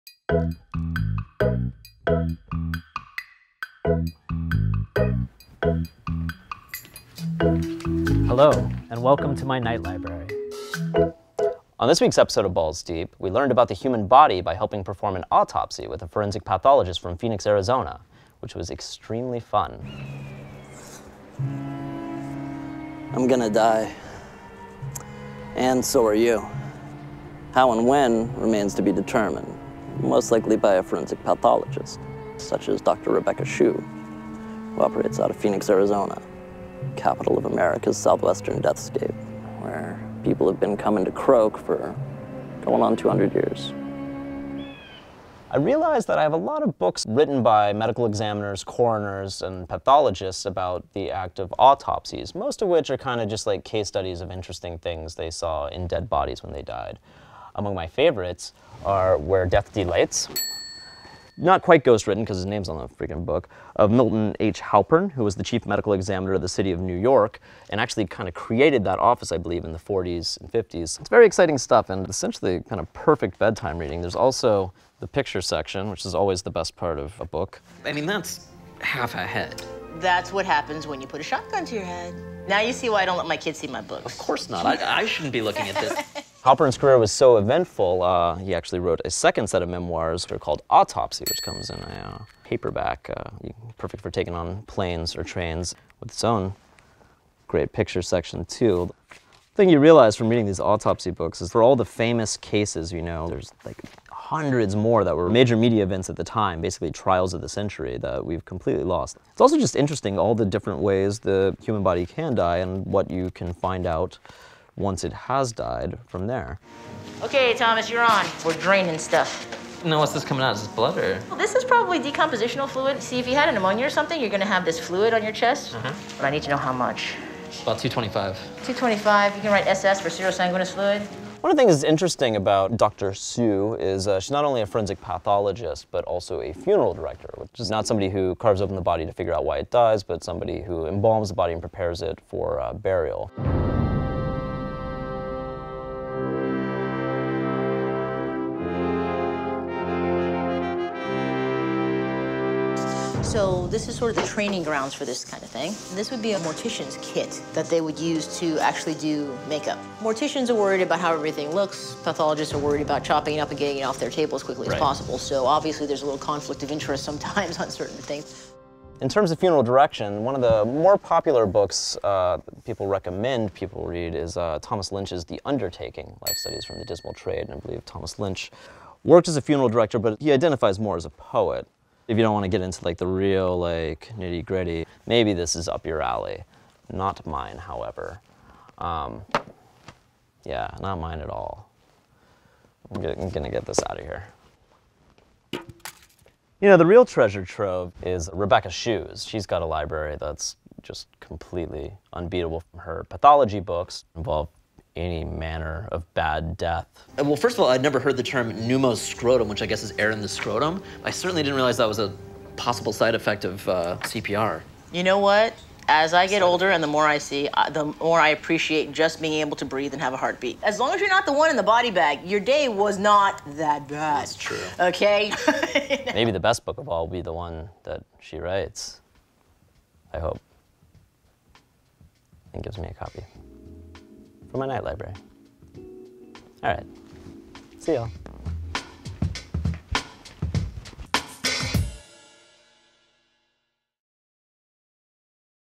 How Thomas Morton Prepared to Face Death
Once again, 'BALLS DEEP' host Thomas Morton brings us into his library to show us the books he read to prepare himself for being around dead people. Before assisting Dr. Rebecca Hsu in performing an autopsy, Thomas read one of his favorite books on the body and death, 'Where Death Delights,' by Marshall Houts, which was written about Milton H. Halpern—the chief medical examiner of New York City in the 50s. Additionally, he read Halpern's second set of memoirs, 'Autopsy,' by Bernard Knight, which covers all the different ways the body can die and everything you can find out about it once it has.